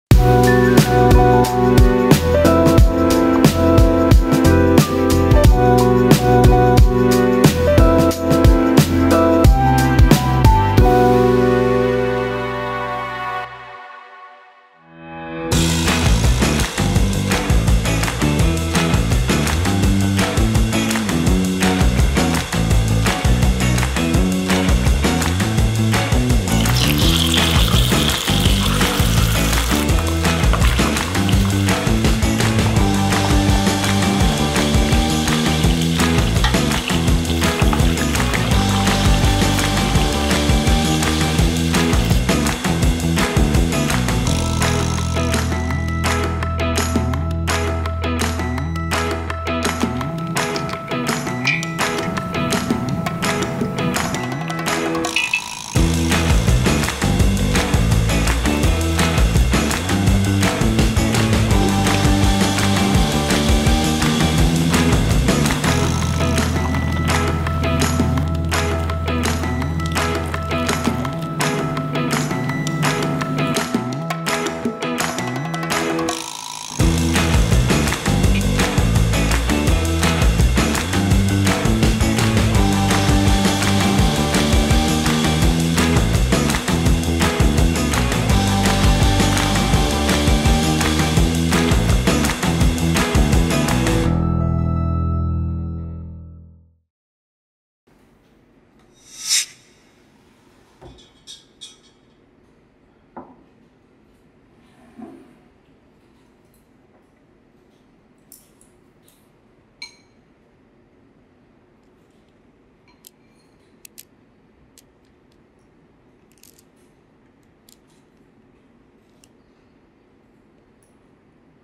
Milkfish and Green Papaya Soup - 遮目鱼青木瓜汤 | Confinement Food Recipes
Here's a recipe that tastes so light and clean yet flavourful at the same time. Click on the link below to find out more!

One of the many confinement food recipes, Milkfish and Green Papaya Soup Chinese-Style boosts the production of breast milk for mothers who are breastfeeding.

Milkfish also has a less “fishy” taste but still has the same nutrients compared to other fishes. The sweetness from green papayas and red dates, combined with the light fish broth makes a pleasant and hearty soup that you will go second rounds for.